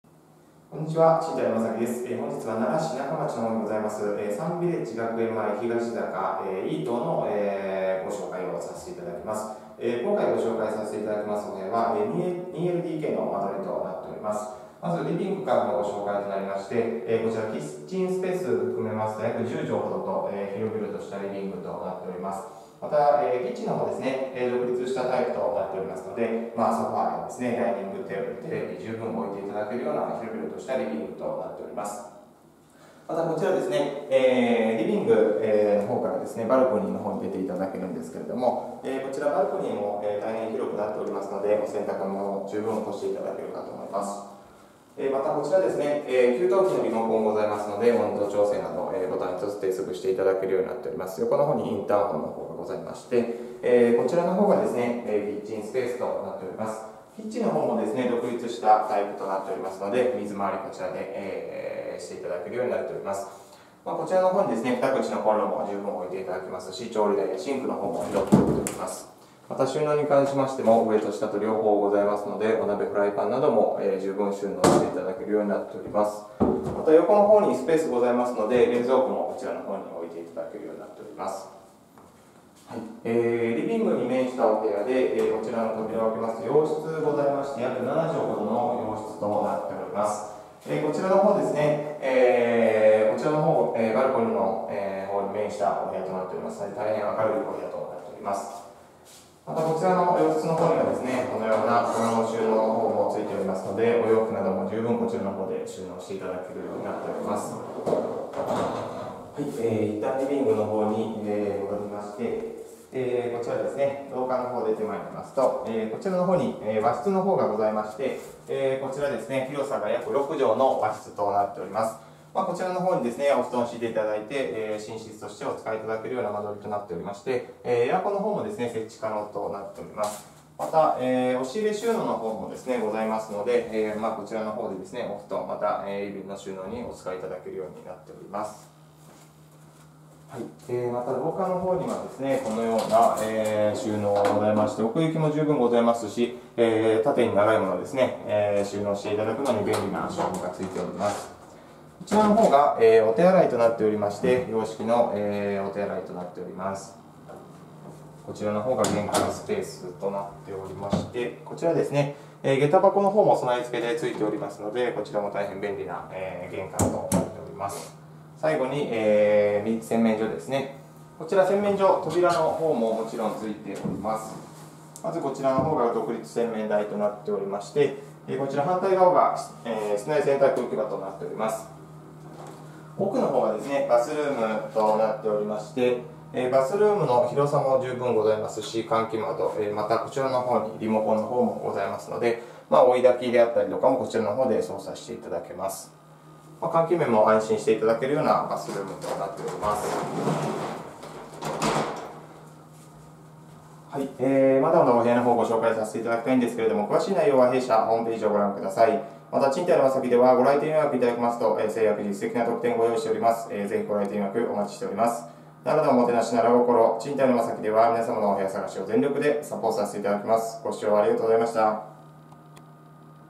9857 サンビレッジ学園前東坂E 201 案内動画♪賃貸のマサキ
奈良市中町にあります２LDKアパートの【サンビレッジ学園前東坂E】の紹介動画になります。

【サンビレッジ学園前東坂E】の詳細情報はこちら